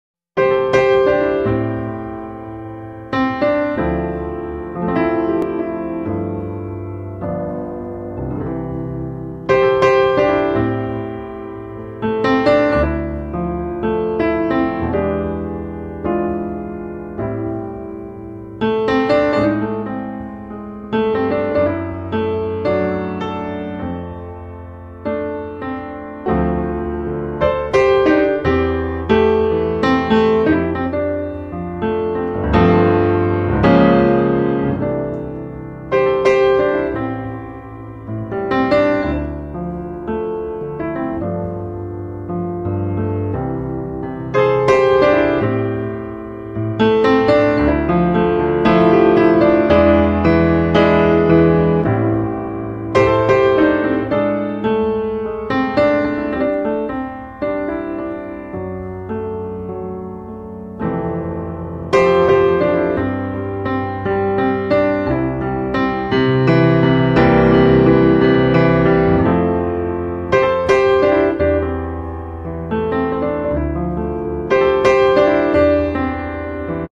Golden Retriever Puppies and Relaxing Piano Music
What could be better than Golden Retriever puppies playing with relaxing piano music in the background